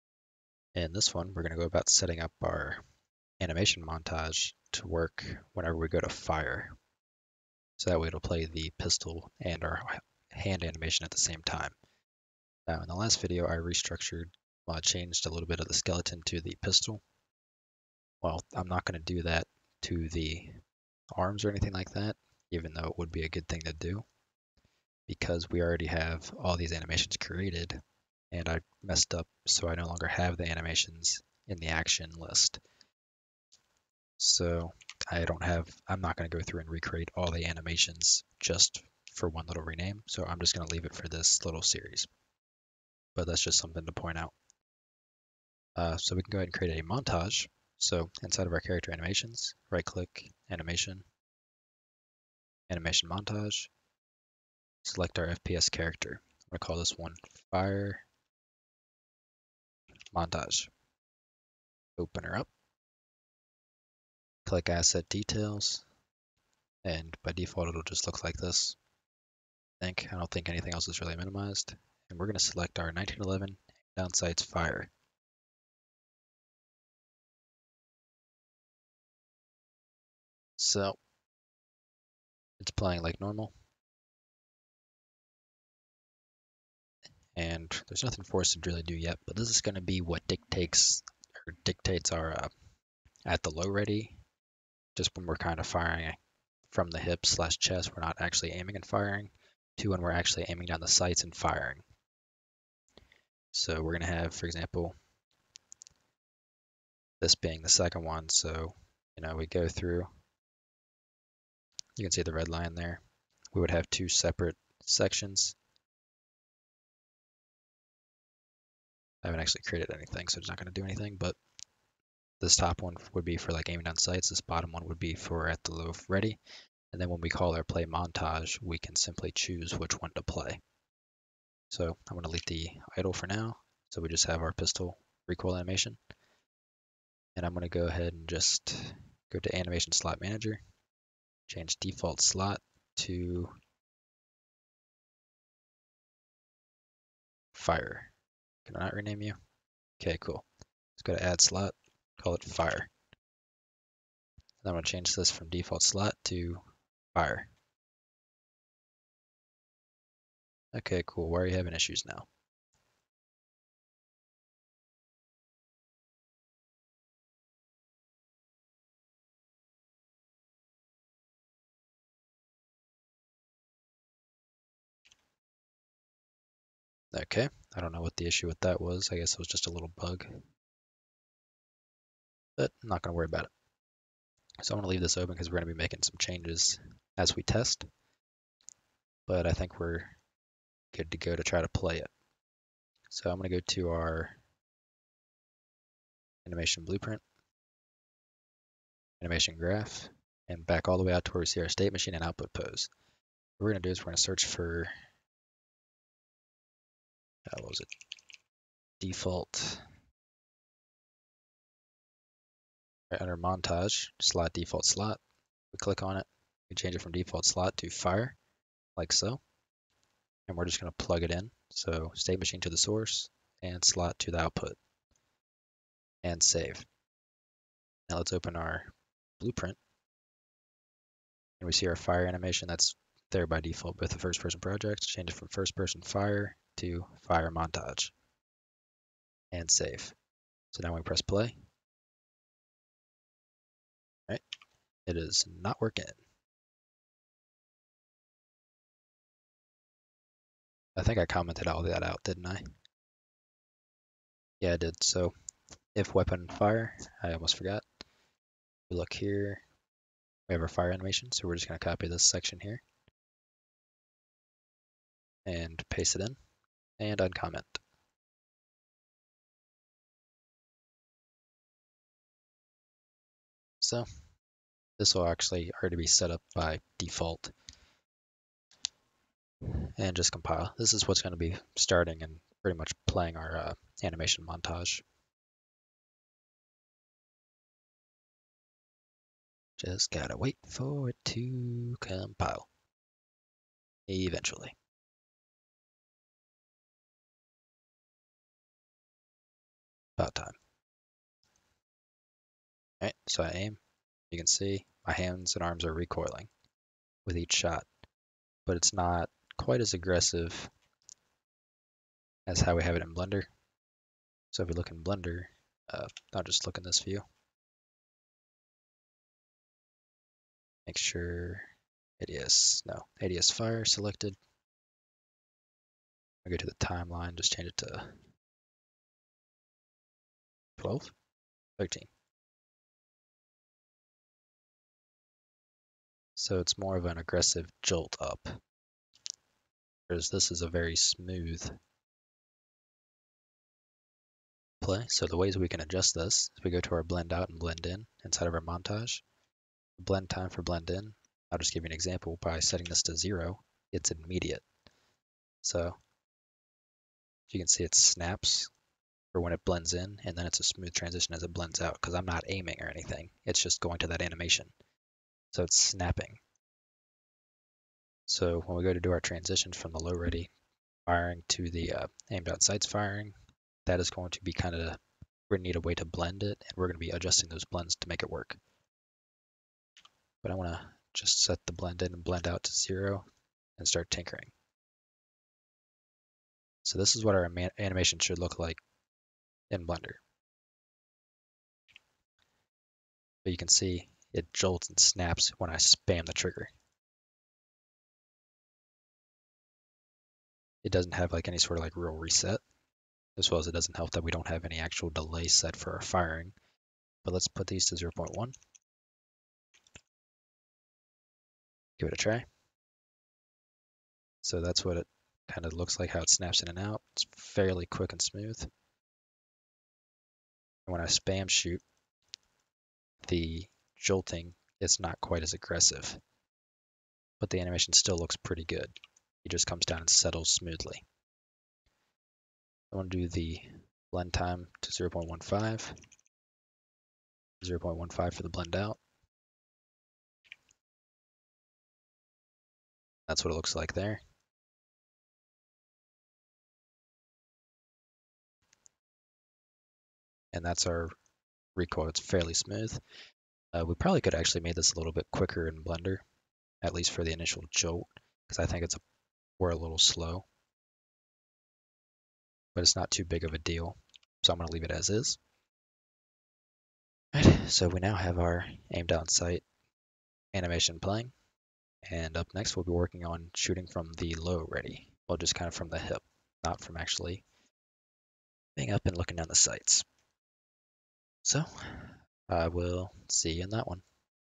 Blender 2.8 FPS animation to UE4 Part 18: "Shooting Animation Montage setup"
In this video we setup an Animation Montage to work with our shooting animation(s).